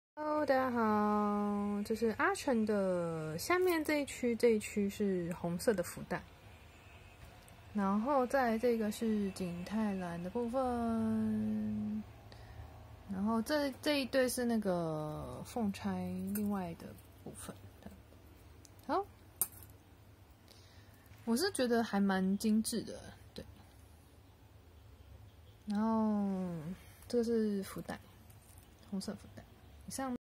[開箱文]開箱髮簪.福袋，及髮夾等飾品
淘寶：阿成的手工坊
包含瓔珞.福袋.髮簪.景泰藍套裝。

海螺天星SkyStars